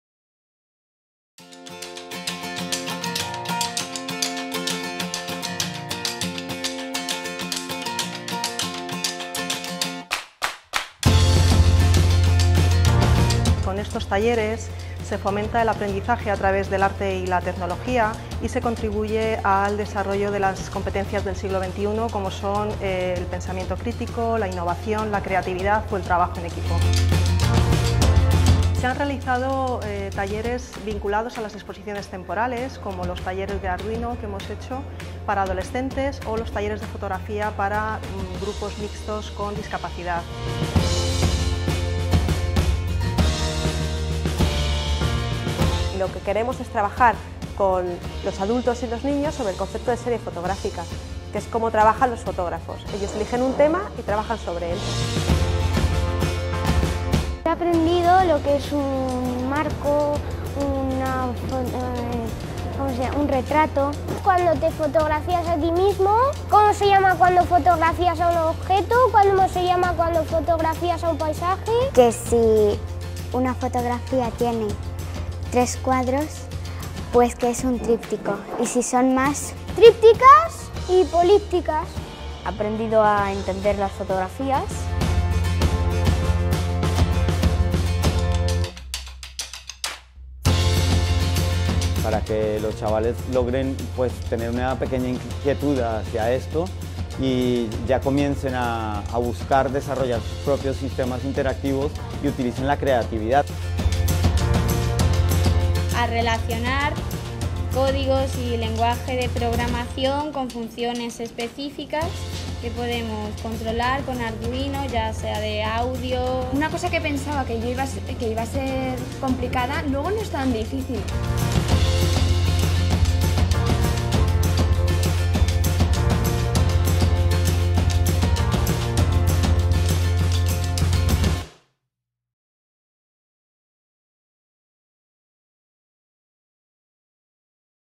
Talleres educativos en el Espacio Fundación Telefónica
Fundación Telefónica desarrolla una serie de talleres dirigidos a los niños, los jóvenes y el público en general. Todos los talleres tienen lugar en Espacio Fundación Telefónica, calle Fuencarral, 3.